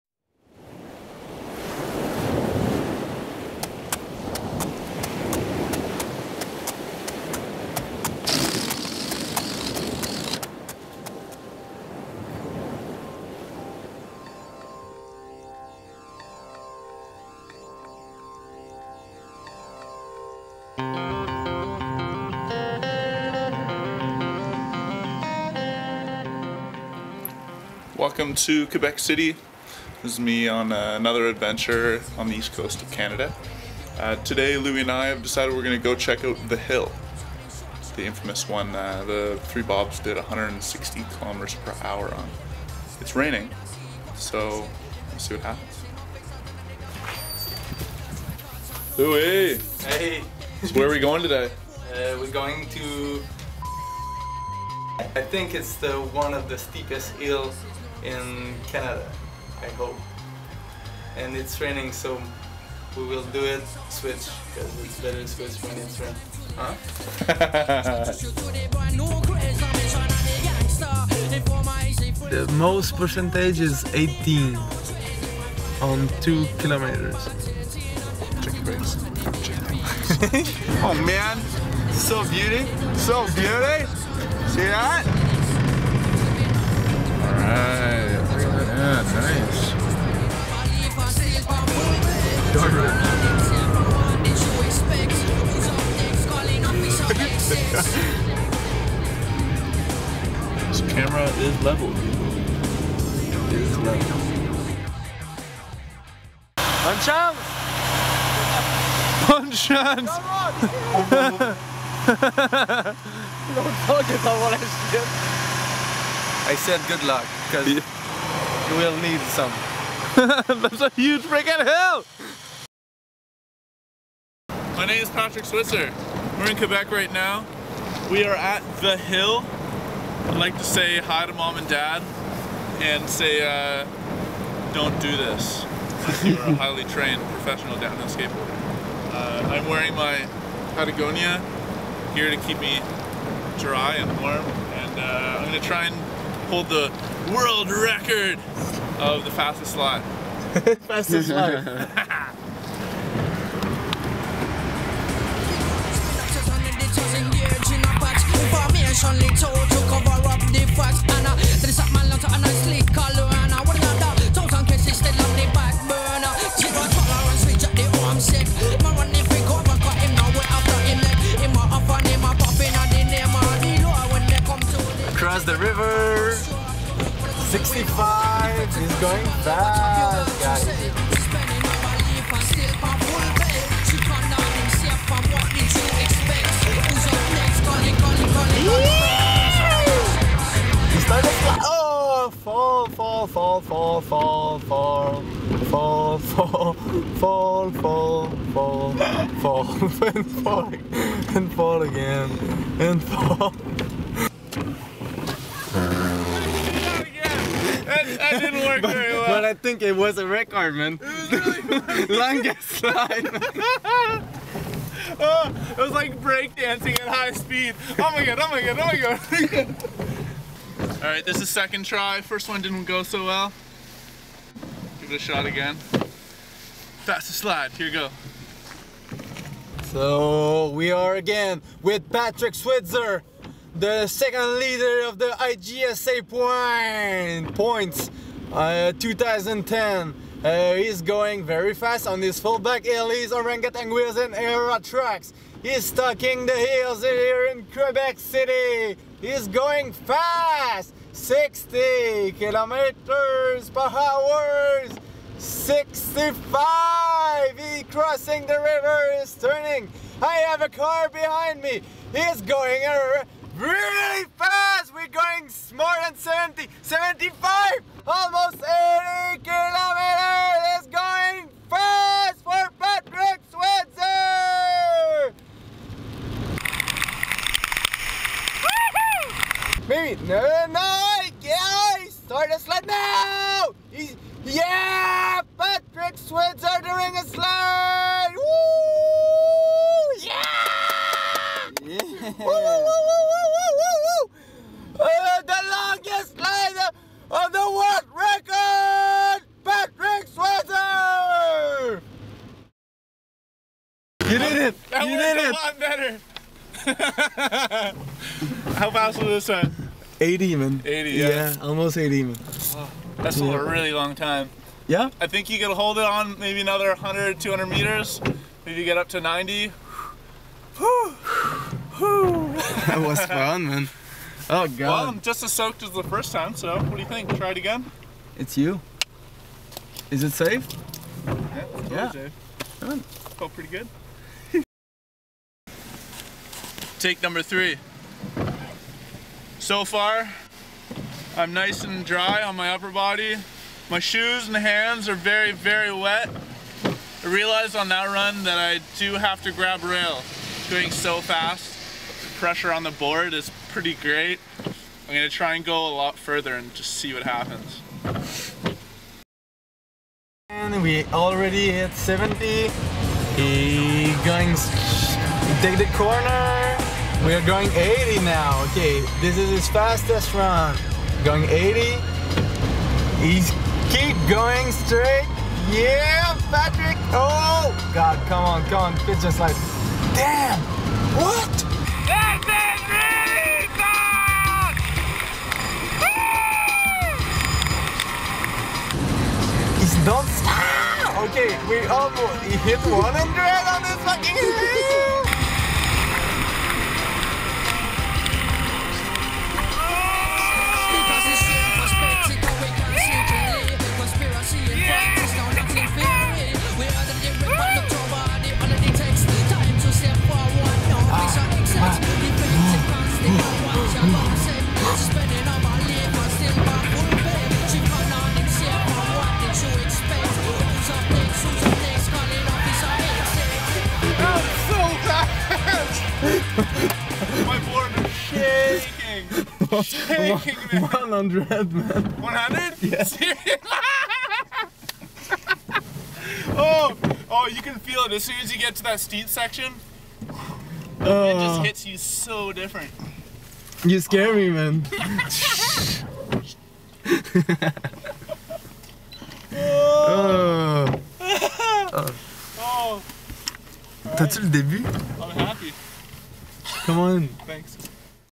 Patrick Switzer World Record Fastest Longboard Slide 100km/hr
The location of this hill has been blocked out for your own safety.  Always remember, when you travel with a skateboard, the locals will show you the way.

btw... In regard to the recent world record chasers, going high speeds is more fun sideways.  This video is a "world record" spoof.

P.S.  The car cam footage of the last slide was of the speedo instead of me...  too bad eh!  The helmet cam footage justifies the intensity, but the clip was from the slide before.  Still gnarly at 8 seconds of sliding.

~ Patrick

Stuntman : Patrick Switzer
Camera : Julien BeGeron, Louis Ricard, Patrick Switzer
Edit : Patrick Switzer
Commentary & Driving : Louis Ricard

Fullbag Dude!